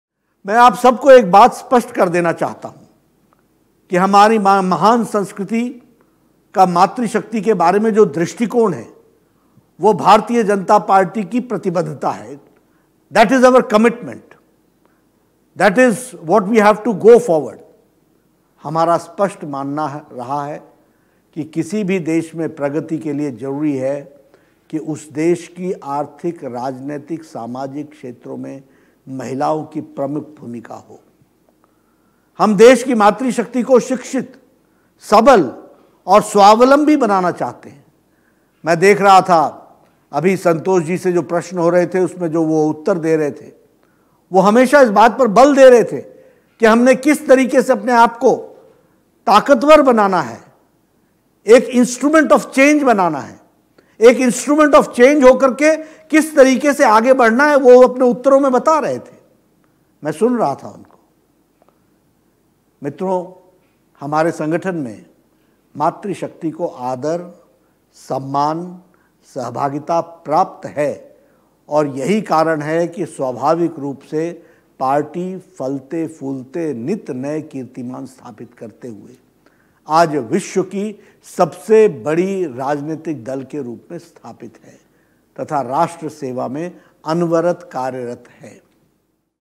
भाजपा में मातृ शक्ति को आदर, सम्मान और सहभागिता प्राप्त है
भाजपा के संगठन में मातृ शक्ति को आदर, सम्मान, सहभागिता प्राप्त है।

यही कारण है कि पार्टी स्वाभाविक रूप से फलते-फूलते नित नए कीर्तिमान स्थापित करते हुए आज विश्व के सबसे बड़े राजनीतिक दल के रूप में स्थापित है, तथा राष्ट्रसेवा में अनवरत कार्यरत है।

- श्री जे पी नड्डा